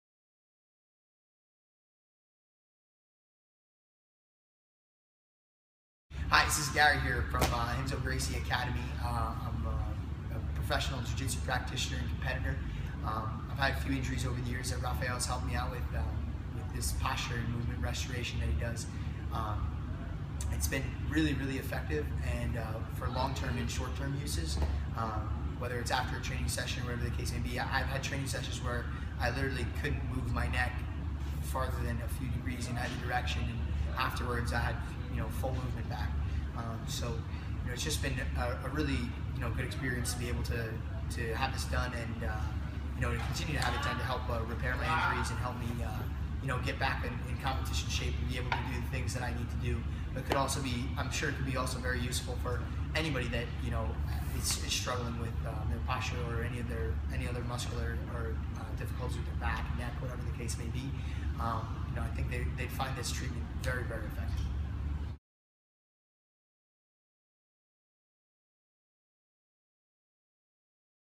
Garry "The Lion Killer" Tonon uses MPR to keep him in fighting shape | Torres Fitness
Professional Grappler Garry "The Lion Killer" Tonnon Uses Movement & Posture Restoration to keep him in fighting shape.

Renzo Gracie Academy NYC
Muscle Activation Techniques
Neurokinetic Therapy
Posturology
Neuromuscular Reprograming